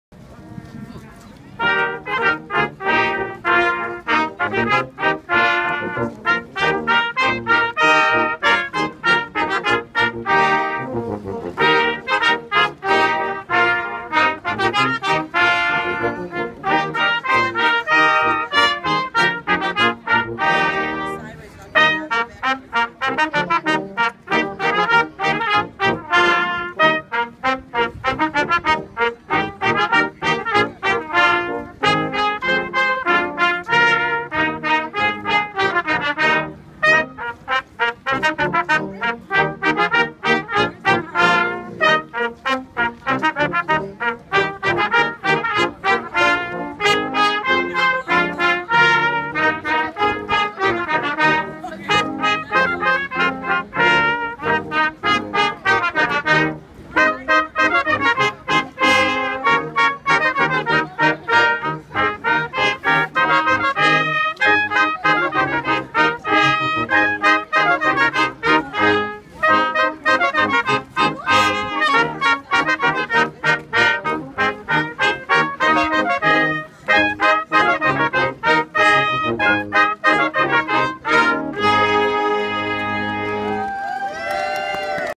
Dodworth Saxhorn Band
Holiday nights in Greenfield Village. Dearborn, Michigan.